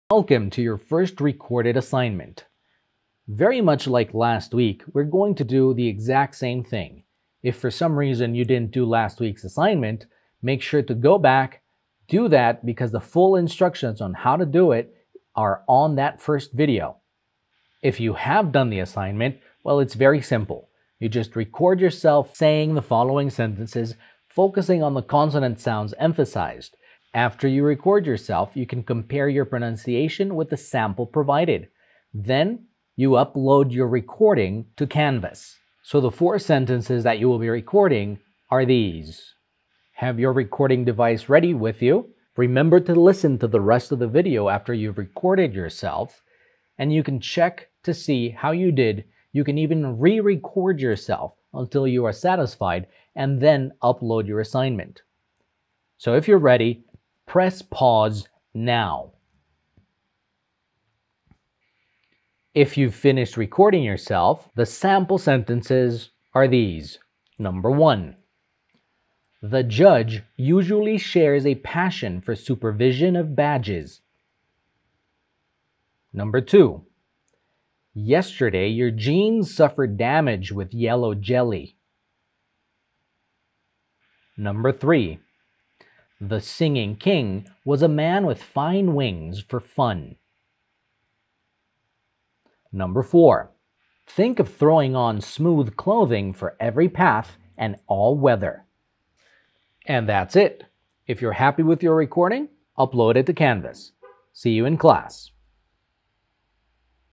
First Recorded Assignment
Recording consonant sounds in sentences.